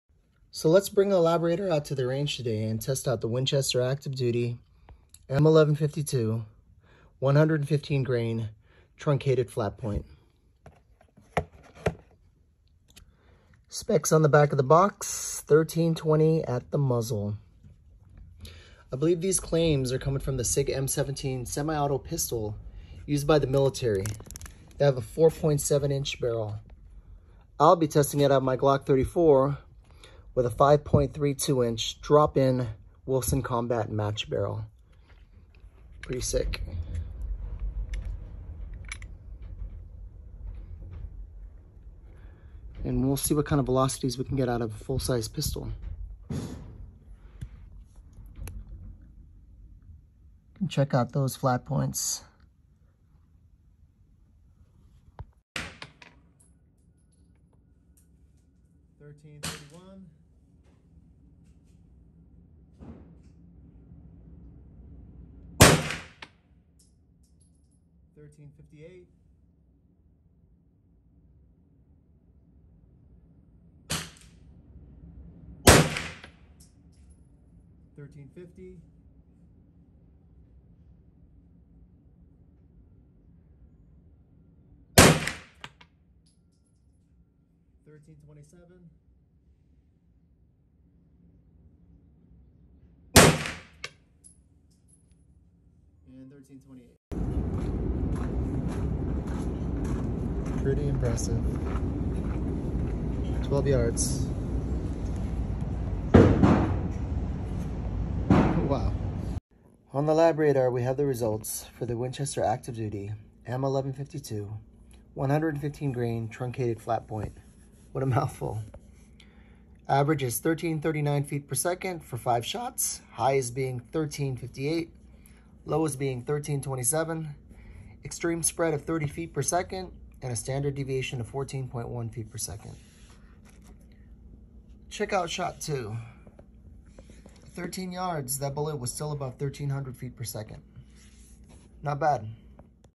M1152 Active Duty 9mm/ LAB RADAR/ G34. "WATCH IN FULL SCREEN"!!
Average velocity of the Winchester M1152 115gr 9mm ammo out of the Glock 34.-( 5 shot average & accuracy @ 12 yards )...